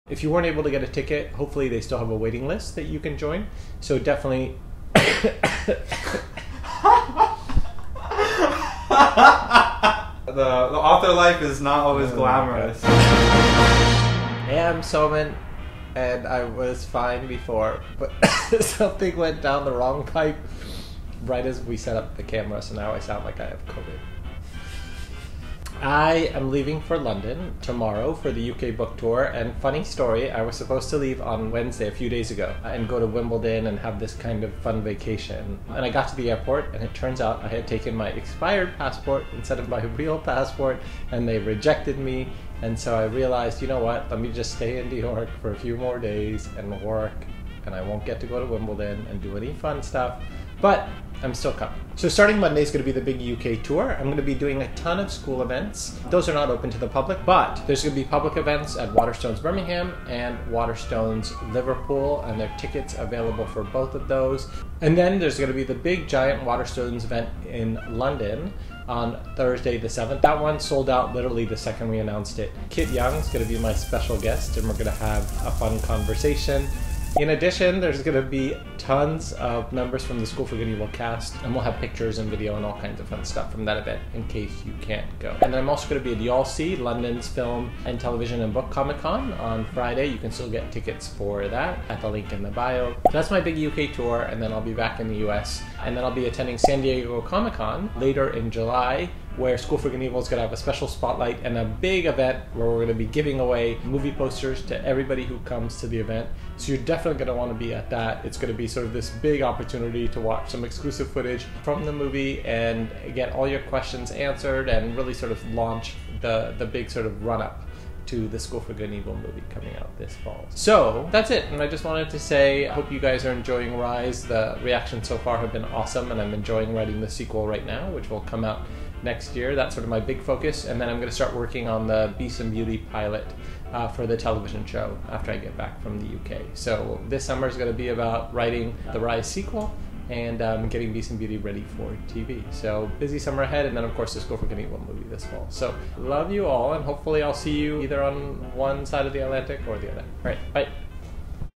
EverNever TV | Soman's Vlog: Summer Good & Evil Tour
Soman checks in before he heads out on his UK Tour, where he'll be re-uniting with the SGE Movie cast and sitting down in conversation with Kit Young (tickets sold out)! Plus, a big SGE spotlight at Comic-Con 2022 in San Diego!

Get your tickets now for the London Young Adult Literature Convention (get your tickets for Friday)

More info to come on the SGE Spotlight Panel!